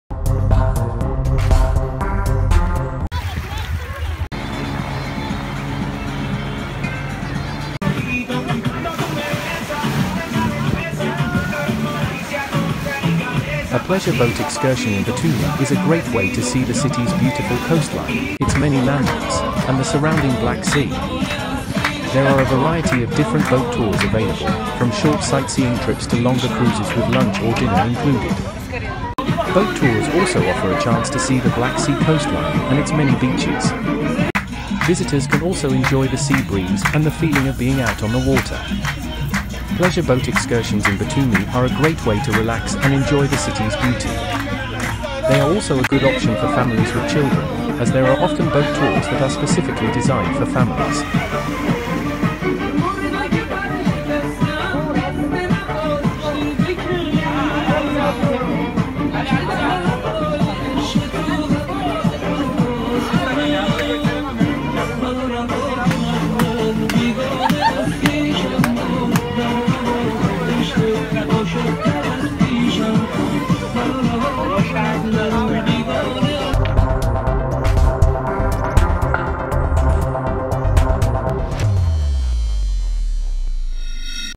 Batumi, Georgia. Pleasure boat excursion
A pleasure boat excursion in Batumi is a great way to see the city's beautiful coastline, its many landmarks, and the surrounding Black Sea. There are a variety of different boat tours available, from short sightseeing trips to longer cruises with lunch or dinner included.

Some of the most popular attractions that can be seen from a boat include:

* The Batumi Arch, a soaring white arch that is one of the city's most iconic landmarks.
* The Alphabet Tower, a unique tower with a spiral staircase that winds its way up to a viewing platform that offers panoramic views of the city.
* The Batumi Boulevard, a long waterfront promenade lined with palm trees, restaurants, and shops.
* The Batumi Dolphinarium, where visitors can see dolphins perform and interact with them.
* The Turtle Lake, a small lake in the heart of the city that is home to a variety of turtles.

In addition to these landmarks, boat tours also offer a chance to see the Black Sea coastline and its many beaches. Visitors can also enjoy the sea breeze and the feeling of being out on the water.

Pleasure boat excursions in Batumi are a great way to relax and enjoy the city's beauty. They are also a good option for families with children, as there are often boat tours that are specifically designed for families.

Here are some tips for choosing and enjoying a pleasure boat excursion in Batumi:

* **Choose a tour that fits your interests and budget.**
* **Bring sunscreen, sunglasses, and a hat, as well as a camera to capture the views.**
* **If you are taking a longer cruise, be sure to pack a lunch or dinner.**
* **Enjoy the scenery and the feeling of being out on the water.**